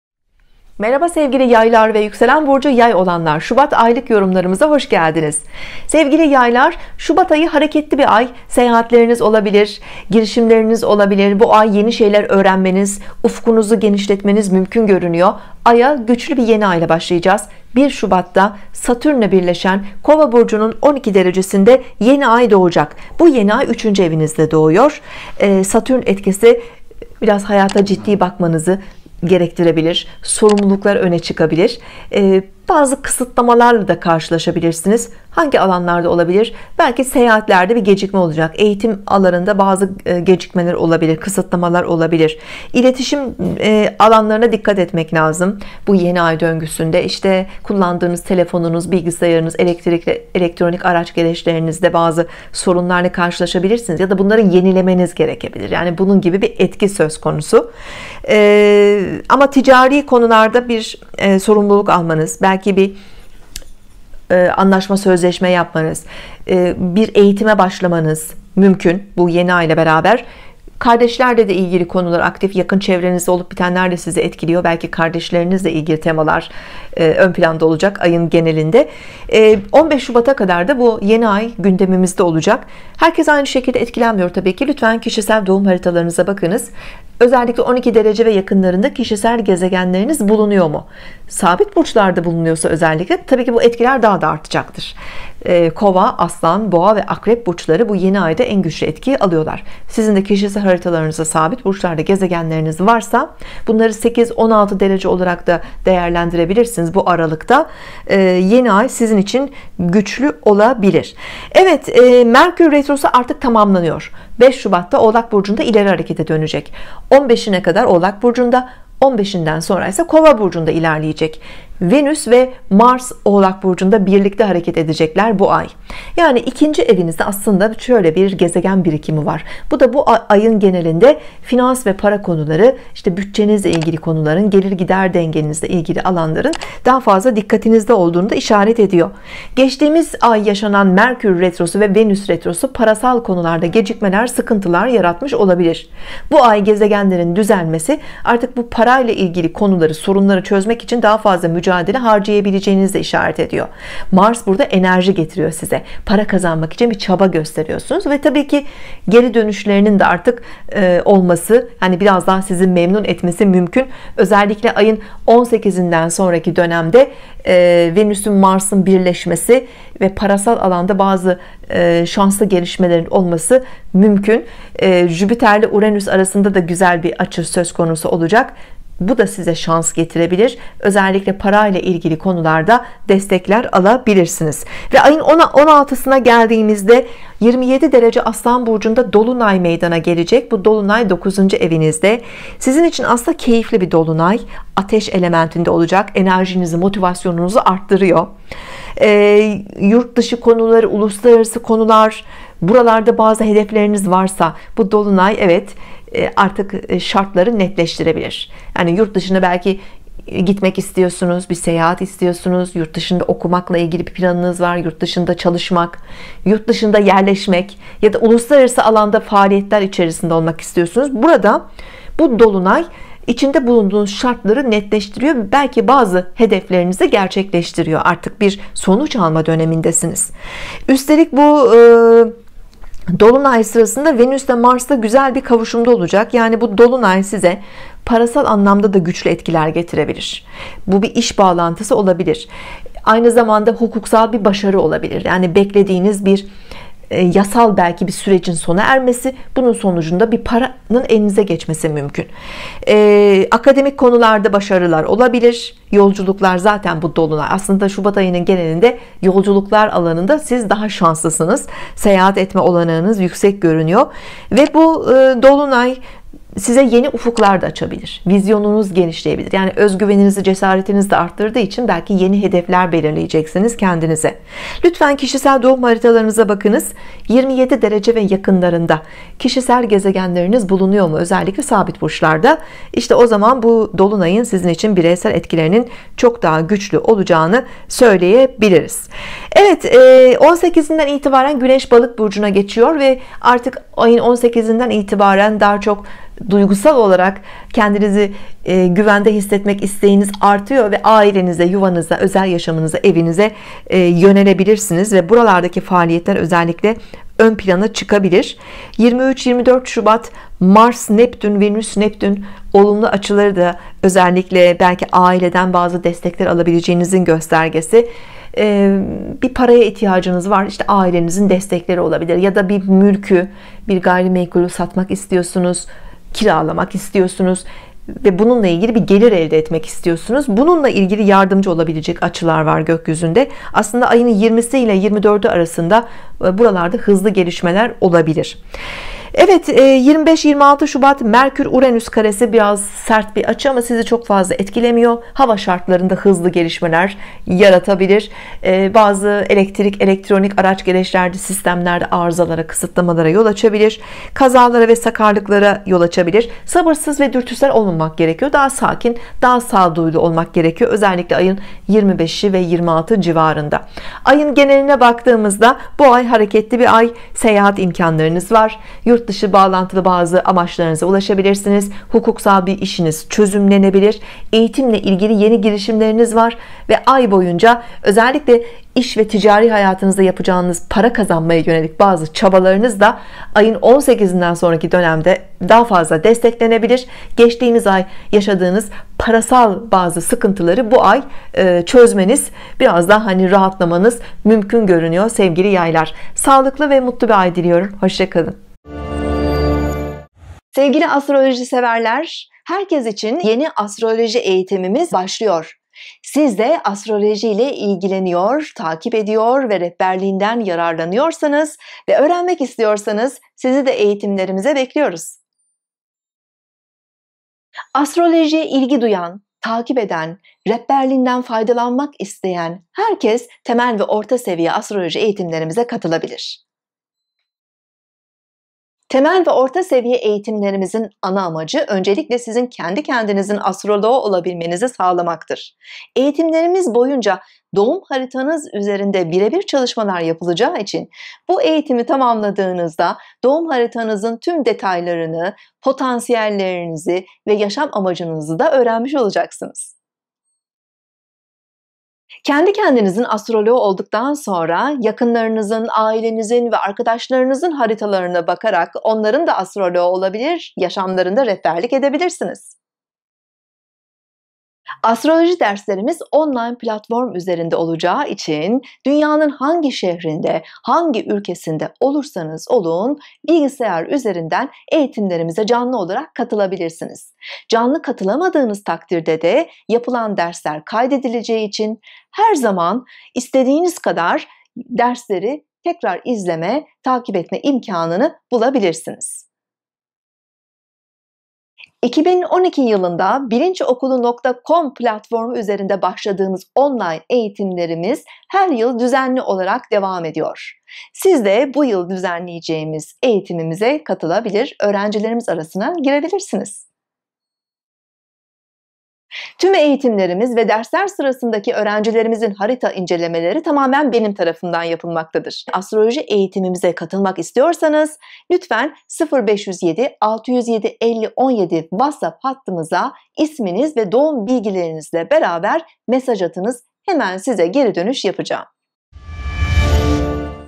Yay Burcu 🔴 Şubat 2022 aylık burç yorumları Demet Baltacı
şubat 2022 aylık yay burç yorumları (yay ve yükselen yay burcu)

yay Burcu ve yükselen yay burcu şubat 2022 sağlık, iş, kariyer ve genel şubat aylık burç yorumları (Astroloji)